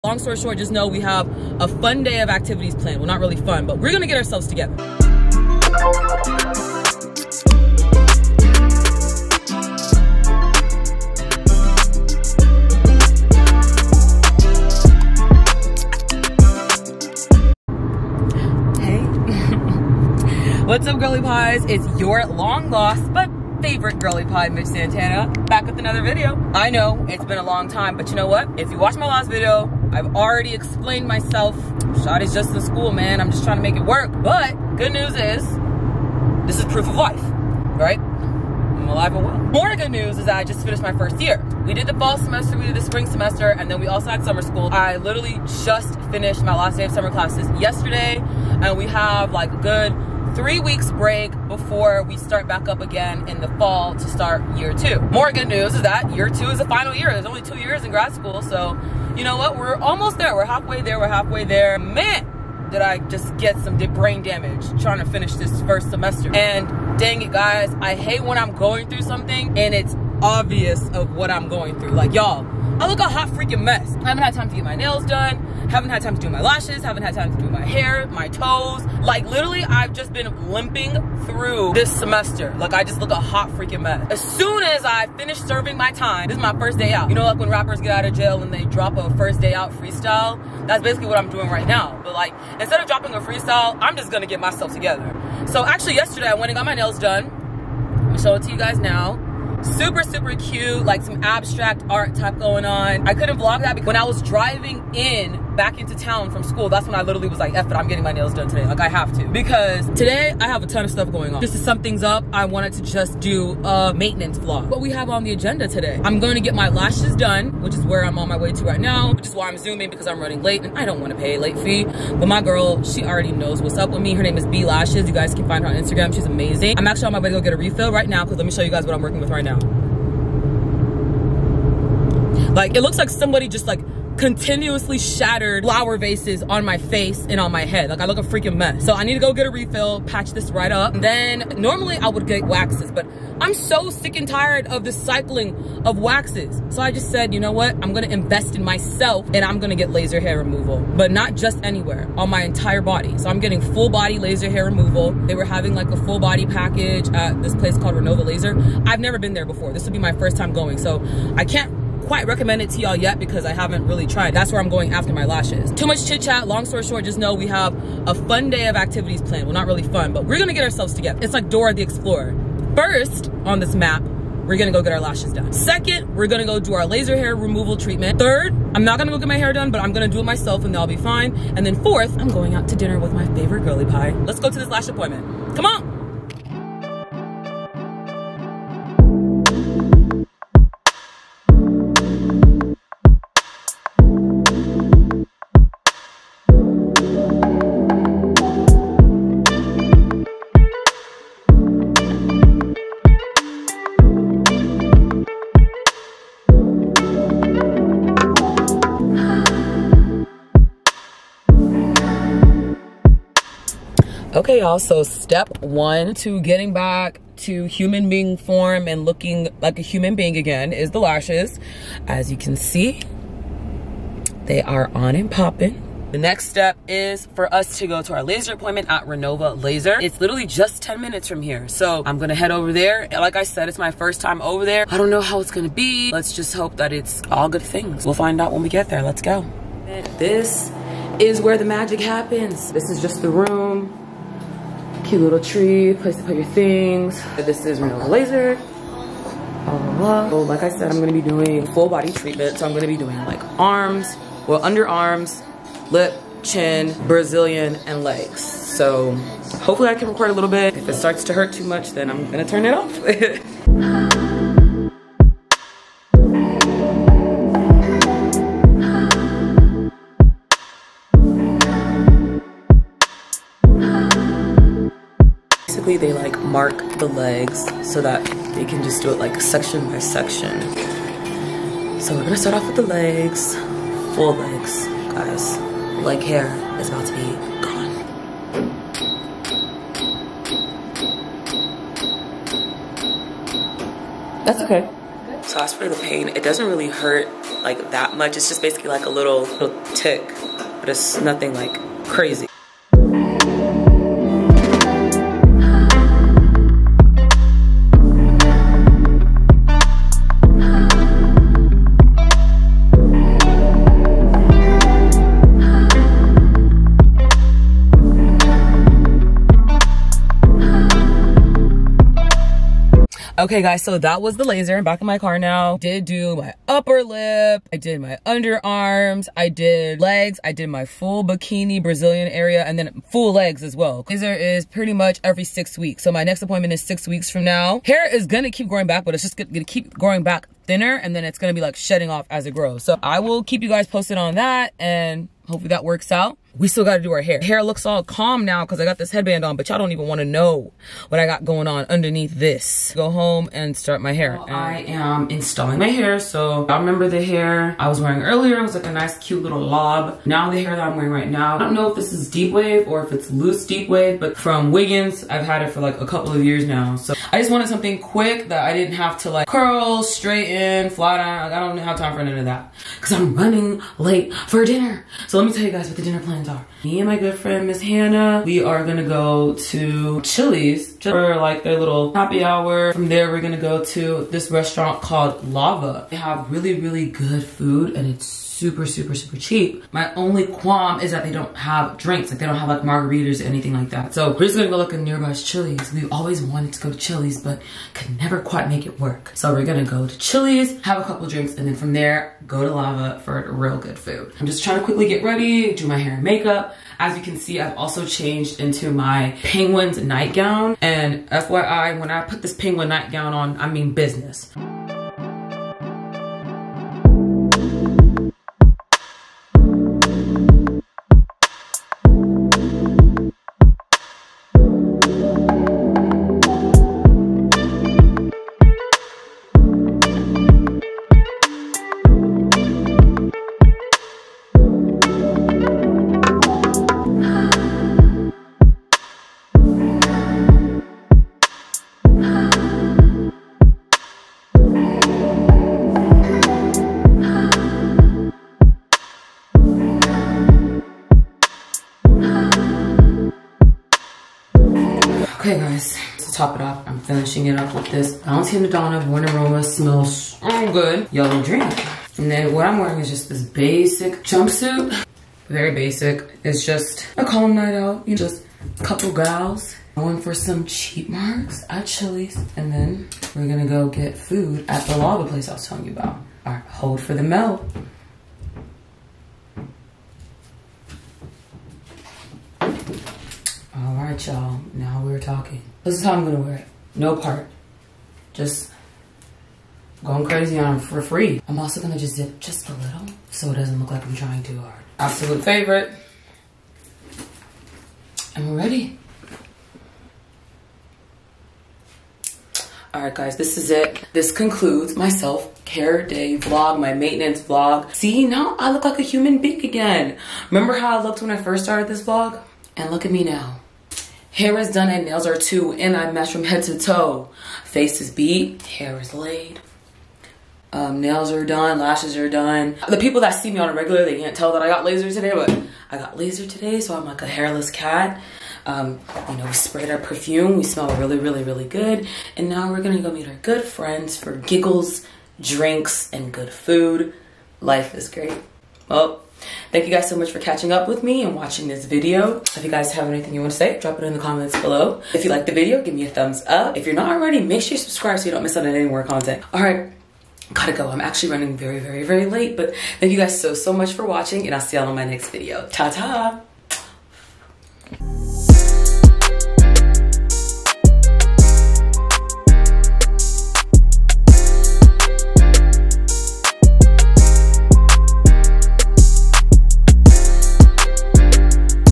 Self Care / Maintenance Vlog | laser hair removal, lashes, nails, etc.
In today's video, I will bring you to all my maintenance appointments. Too much has been going on and I look at hot mess so I will be fixing myself up via lashes, laser hair removal, then doing my hair and makeup so I look like a human being again! Hope ya'll enjoy!

Don't be shy, lets be friends 👁👁

ABOUT ME/FAQ
Age: 25
Birthday: Dec 3rd
Location: Houston TX
Height: 5’7